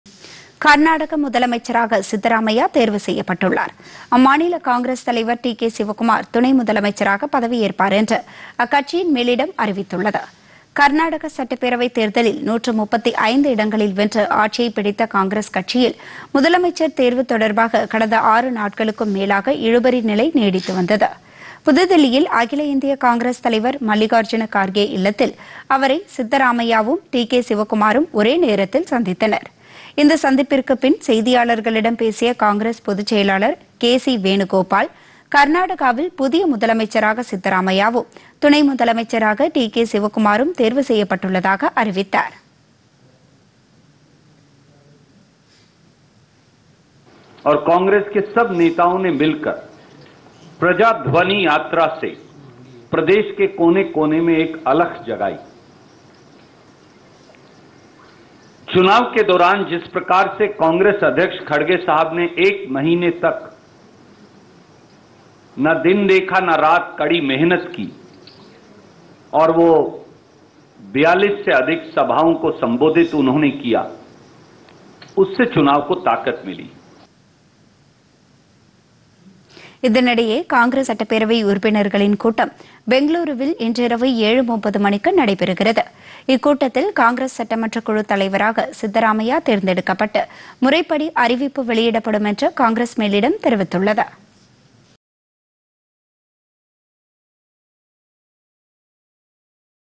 புதிய முதலமைச்சர் சித்தராமய்யா
கர்நாடக முதலமைச்சராக சித்தராமய்யா தேர்வு - துணை முதலமைச்சர் ஆகிறார் டி கே சிவக்குமார் - காங்கிரஸ் மேலிடம் அதிகாரப்பூர்வ அறிவிப்பு